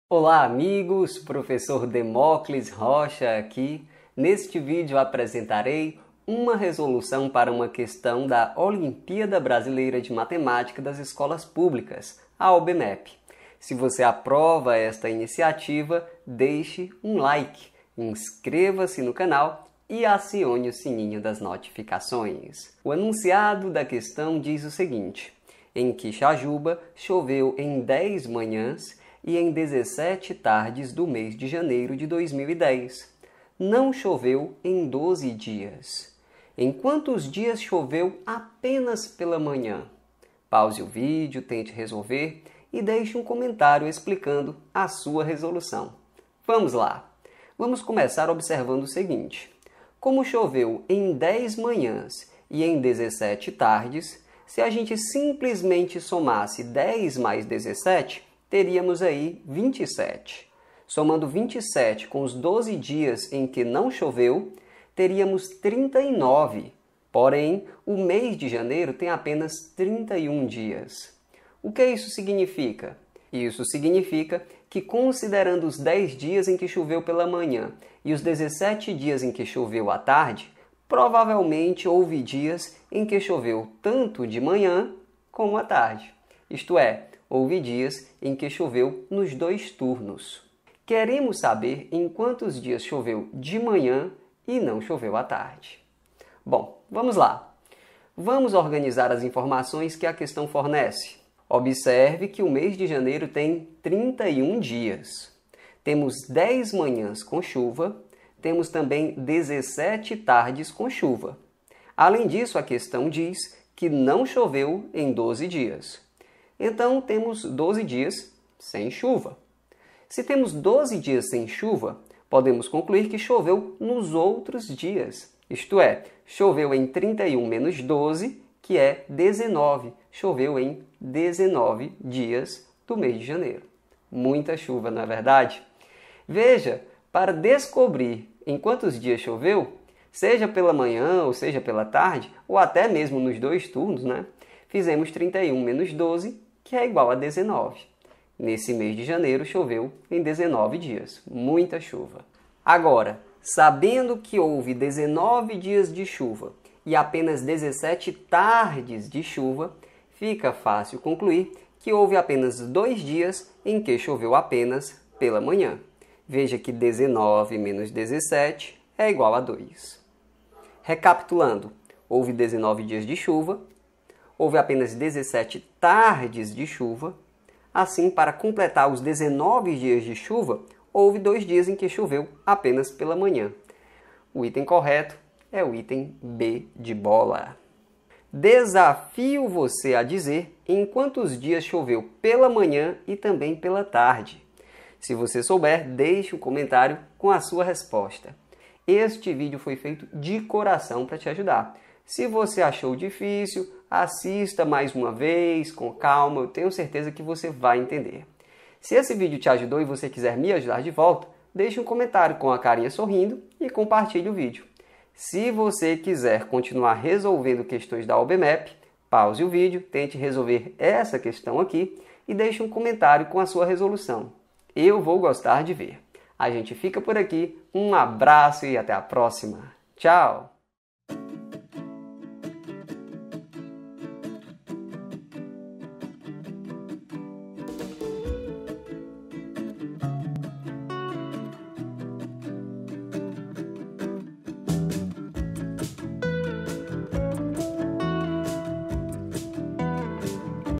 OBMEP - RACIOCÍNIO LÓGICO
Olá, amigos do meu canal no YouTube. Eu sou o professor Demóclis Rocha e, neste vídeo, você vai ver uma resolução para uma questão da Olimpíada Brasileira de Matemática das Escolas Públicas.

16. Em Quixajuba choveu em 10 manhãs e em 17 tardes
do mês de janeiro de 2010. Não choveu em 12 dias. Em
quantos dias choveu apenas pela manhã?

A) 1
B) 2
C) 3
D) 4
E) 5

CÂMERAS

MICROFONES